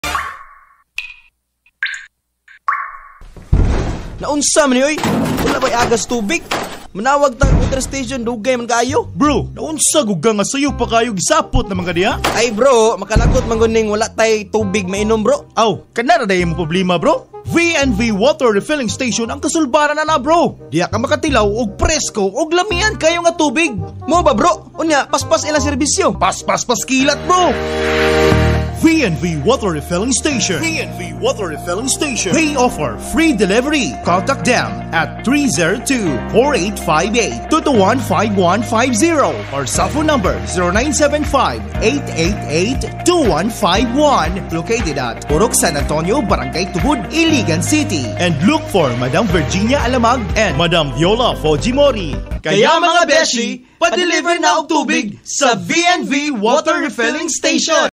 V&V Water Refilling Station Radio Ad 2022 58s (Iligan)
This is the provincial radio commercial for V&V Water Refilling Station in Iligan City.

DRPS Two 2015: V&V Water Refilling Station Radio Ad 2022 58s (Iligan)